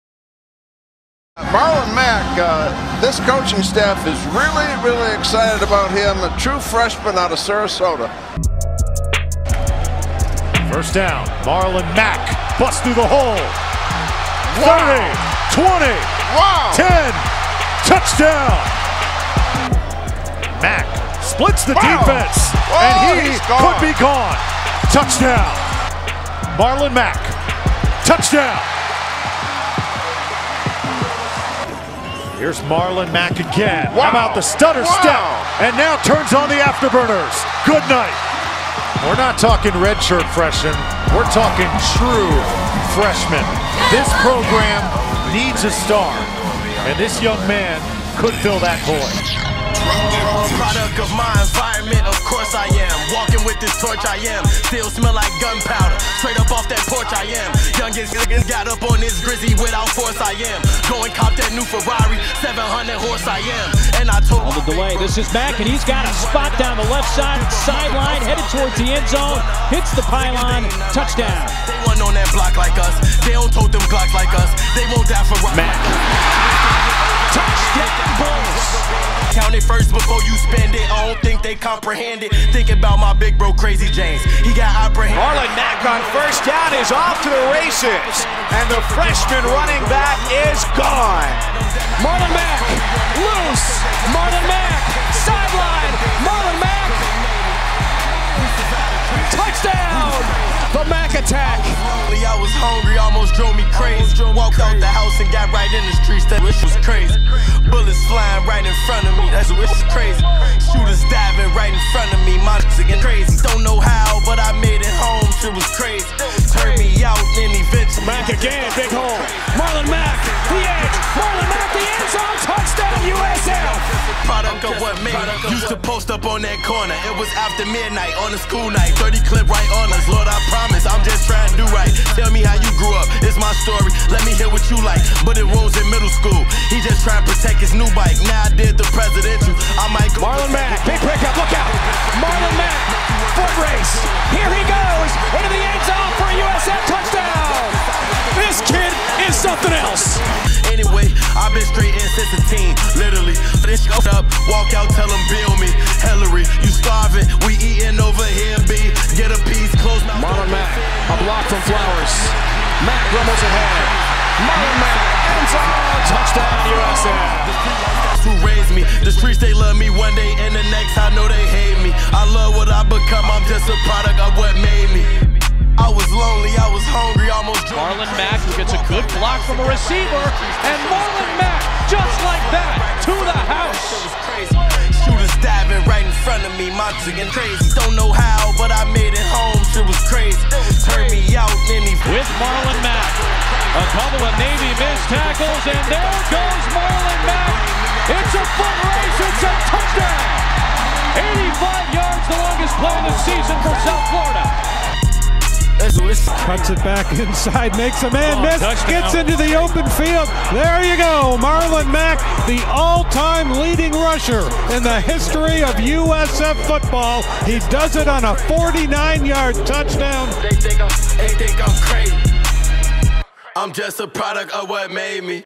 Marlon Mack Official USF Highlight
Highlight for the Mack Attack, Marlon Mack, RB from Sarasota, FL. Mack set basically every rushing record in USF Football history in only 3 years.